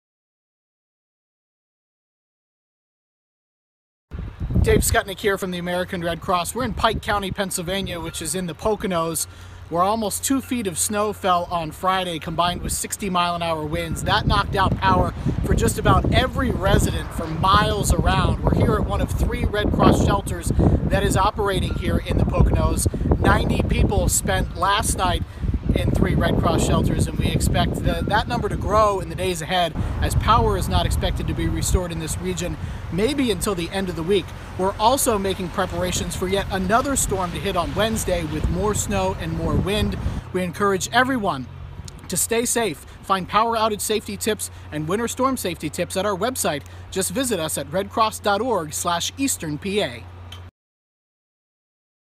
Nor'easter Pocono Update
A massive nor'easter has knocked out power to much of the Poconos with shelters open